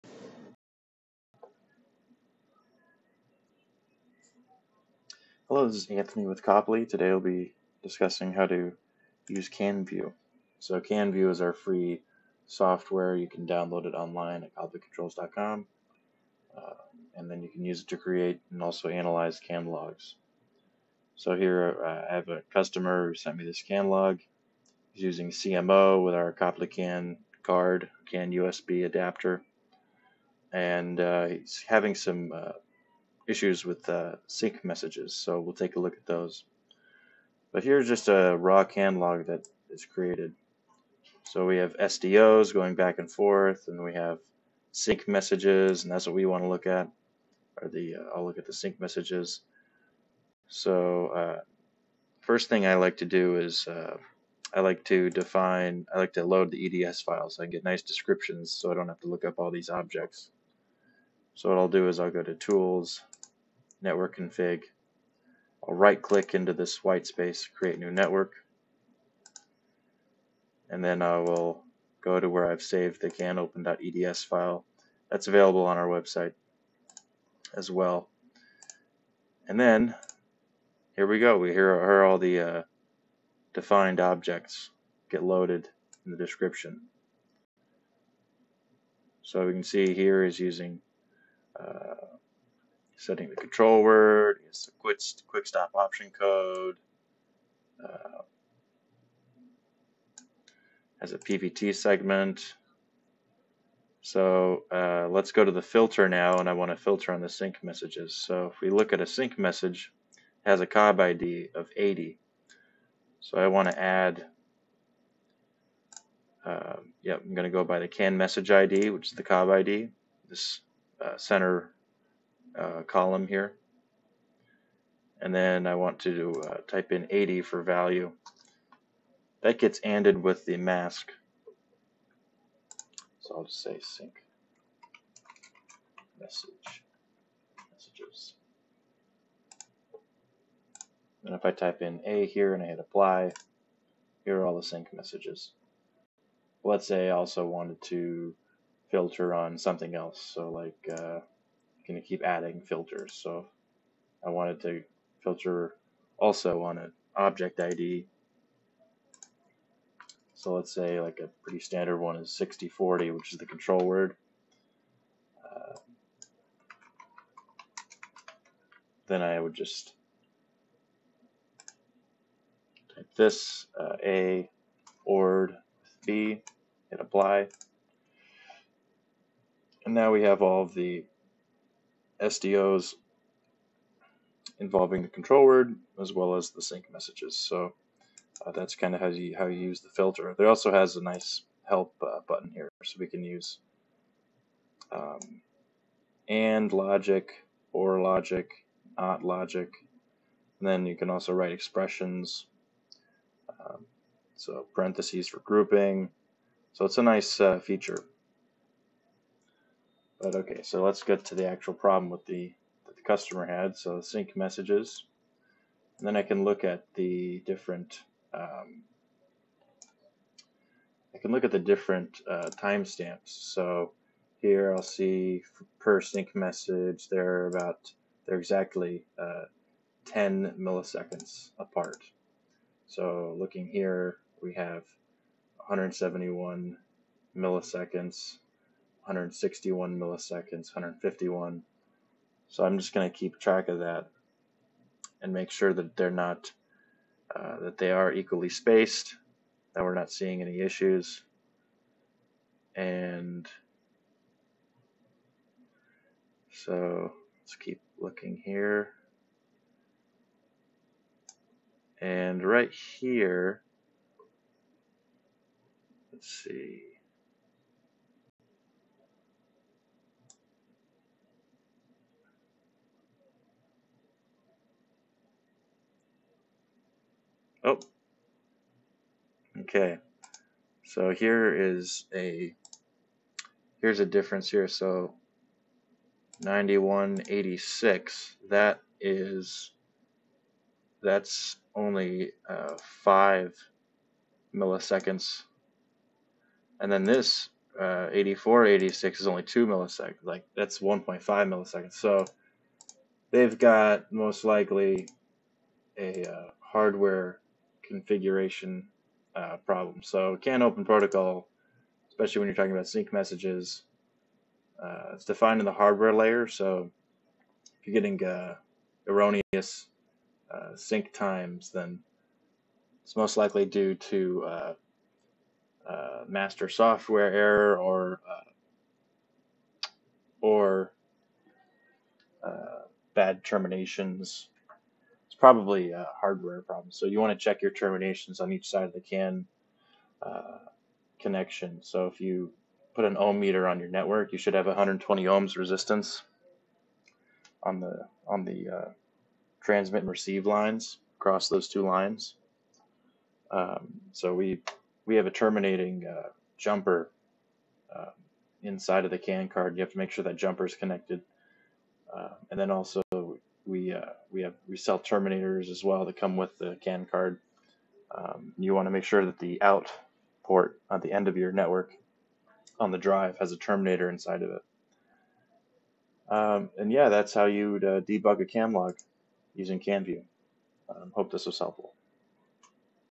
CANview: Free Software to Analyze CAN Message Logs
The following video describes how to use CANview which is our free CAN Network Analysis Software. It can create message logs and also has a useful filter feature. Only compatible with Copley CAN Cards.